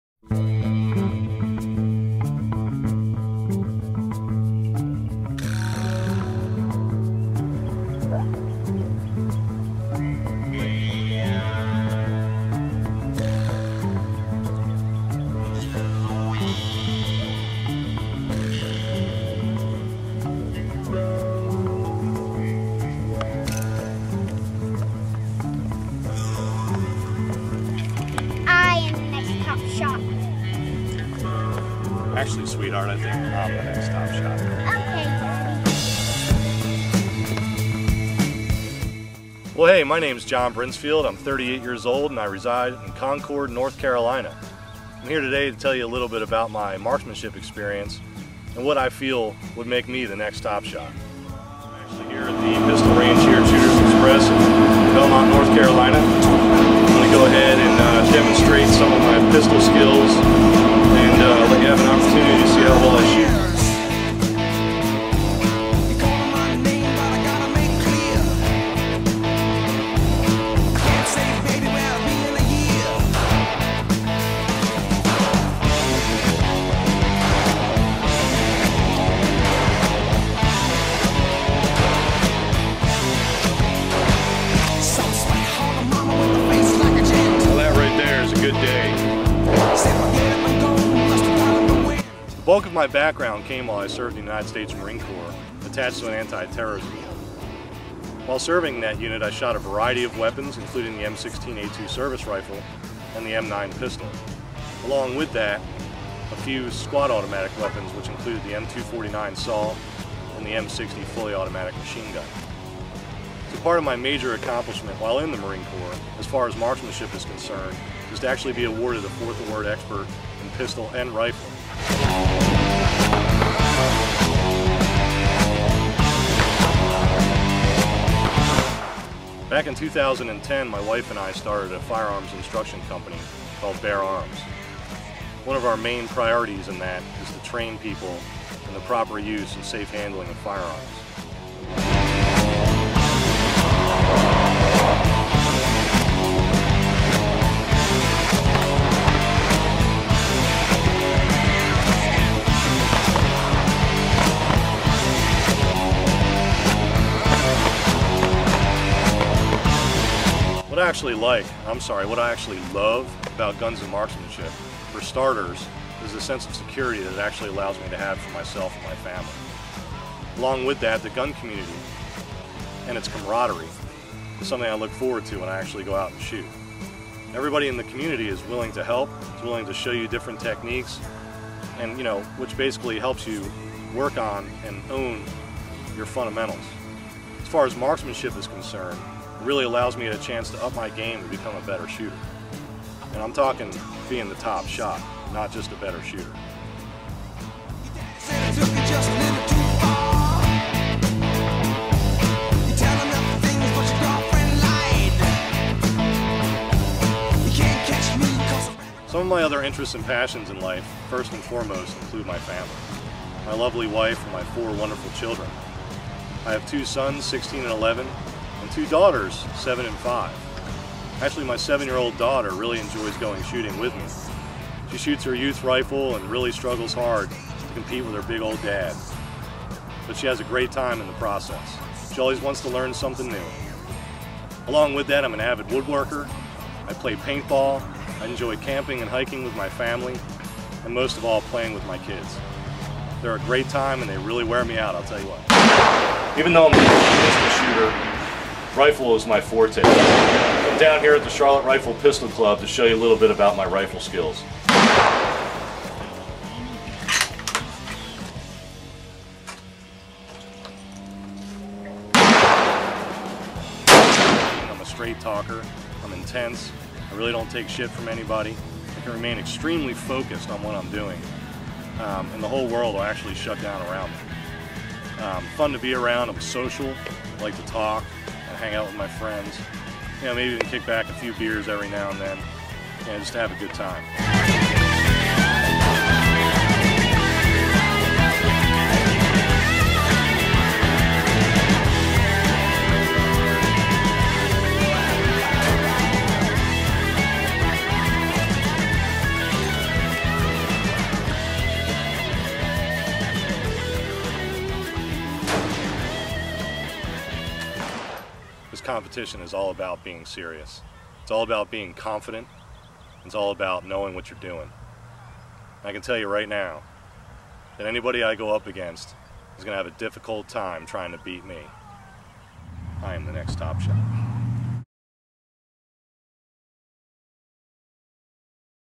John Brinsfield Top Shot Season 4 Audition
John Brinsfield's Top Shot Season 4 audition. I'm looking forward to the opportunity!